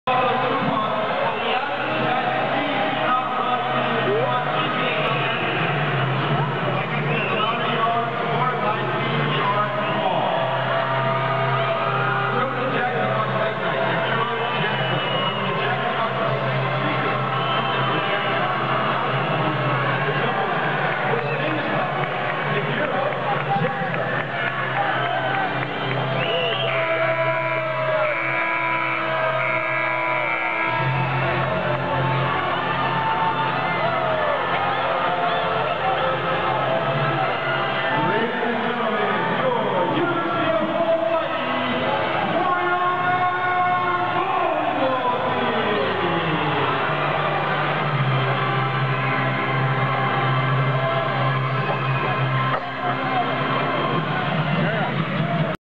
University of Hawaii / Jack-in-the-Box Airship dropping coupons
click2ed videos

Hawaii Warriors vs Pacific Tigers Men's Volleyball
UH Stan Sheriff Center - March 22, 2013